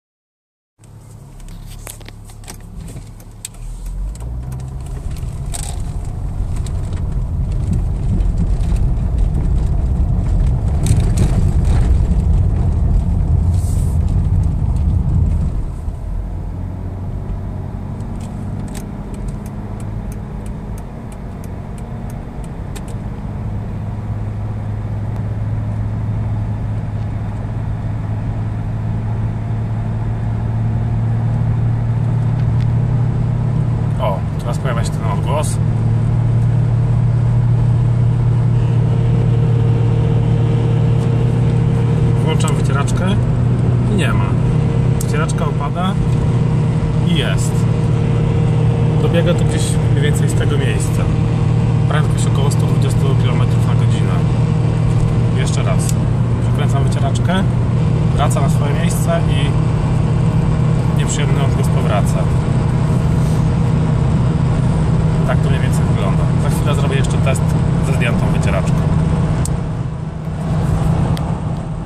Mercedes 210 kombi po wymianie przedniej szyby I
Powyższe nagranie prezentuje nieprzyjemne odgłosy i drgania dochodzące z okolic słupków i podszybia, które pojawiły się po wymianie przedniej szyby.